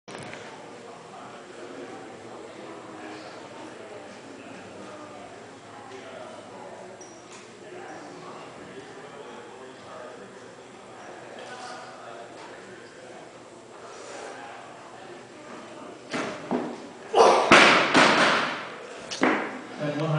100kg snatch miss
3rd attempt. 2011 Jack Frost Open